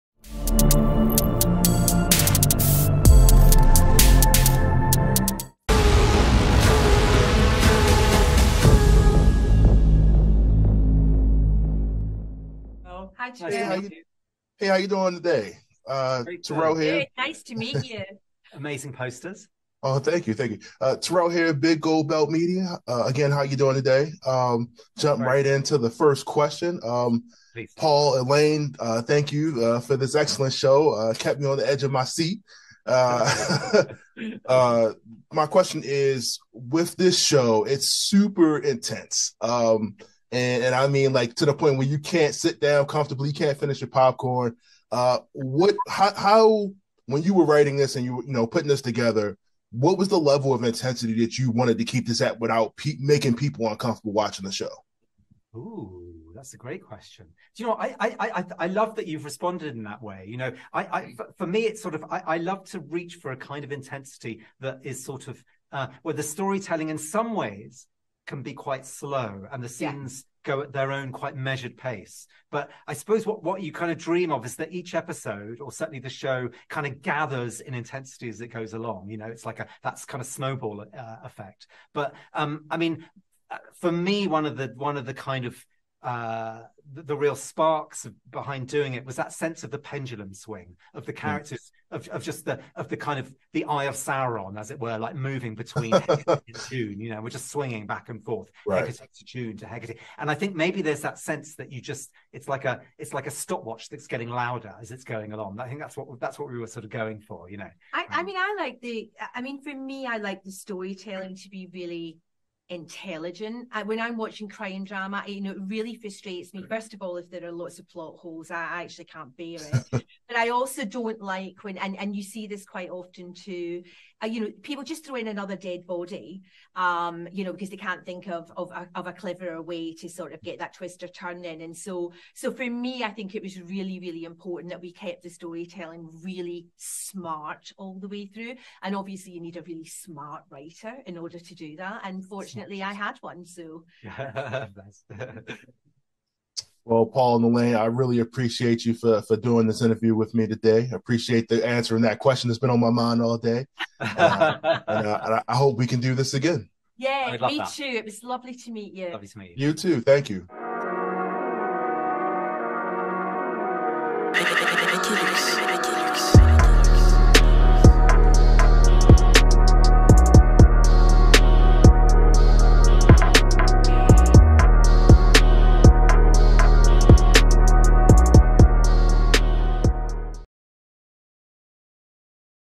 Paul Rutman & Elaine Collins Interview | Apple TV+ Criminal Record
Paul Rutman & Elaine Collins Interview by Big Gold Belt Media

Paul Rutman (Writer, Executive Producer) & Elaine Collins (Executive Producer)
Synopsis:
From BAFTA Award nominee Paul Rutman (“Vera,” “Indian Summers”), Criminal Record,” is a powerful new London crime thriller starring Academy Award winner Peter Capaldi and Cush Jumbo. An anonymous phone call draws two brilliant detectives into a confrontation over an old murder case — one a young woman in the early stages of her career, the other a well-connected man determined to protect his legacy. The series touches on issues of race, institutional failure and the quest to find common ground in a polarized Britain.